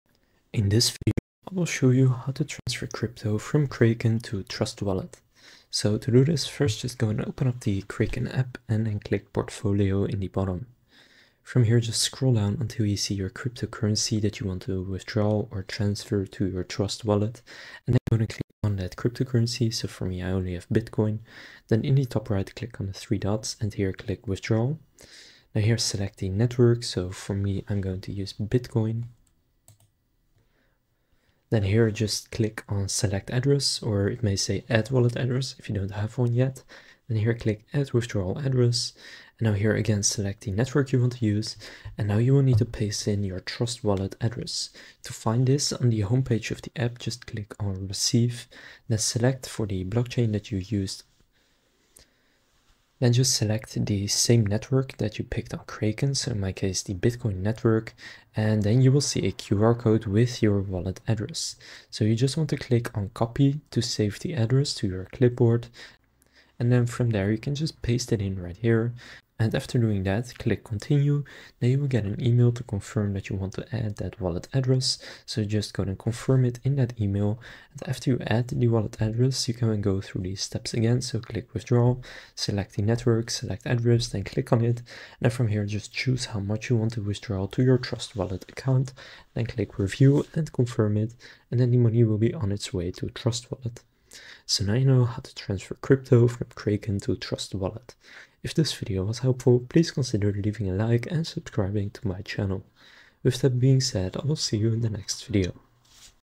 How to Transfer Crypto From Kraken to Trust Wallet (2026)
In this video I'll show you how to Transfer Crypto From Kraken to Trust Wallet.